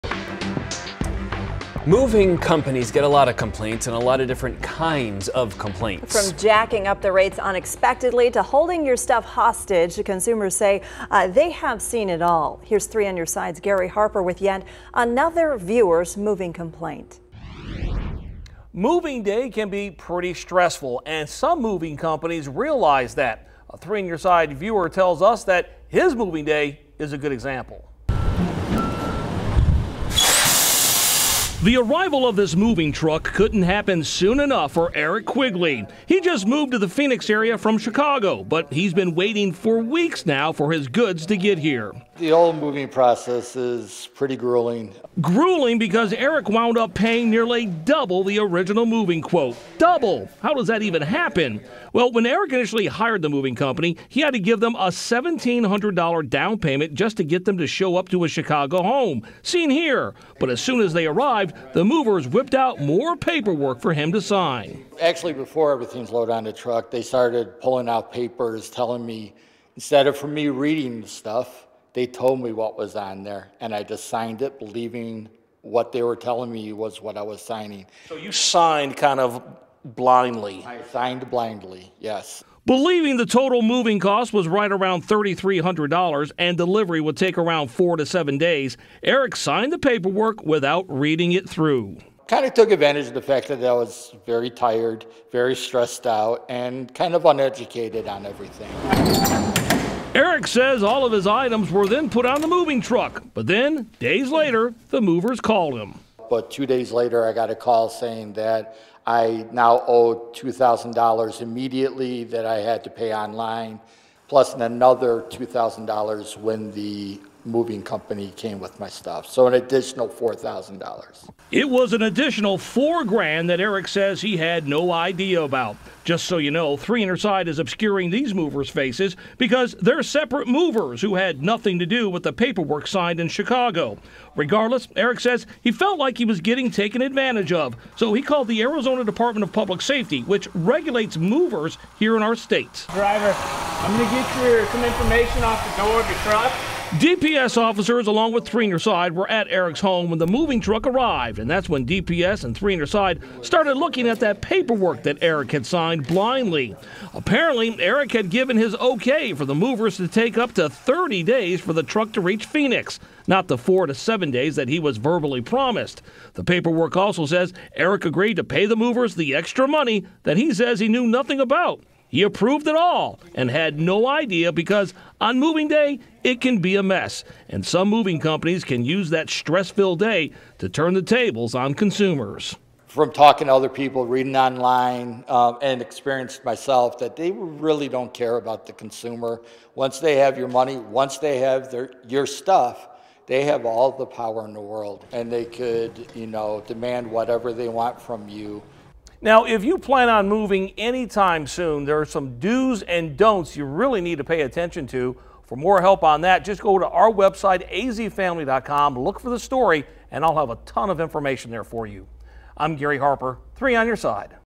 Peoria man says he had to pay double for cross-country move
A Peoria man says he feels taken advantage of after signing papers and claiming he had to pay double for a cross-country move.